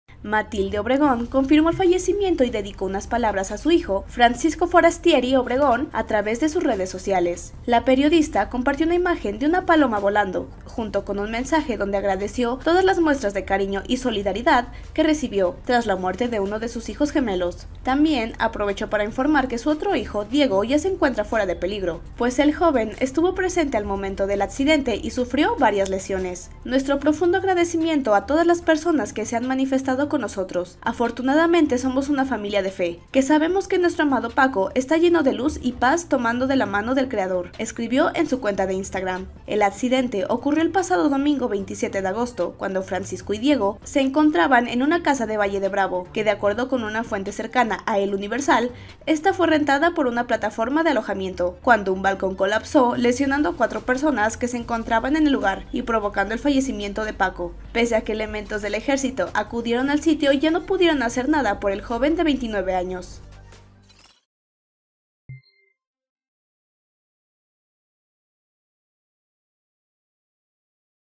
Así se despidió Matilde Obregón de su hijo que falleció en un accidente
La periodista no había confirmado el fallecimiento de su hijo de 29 años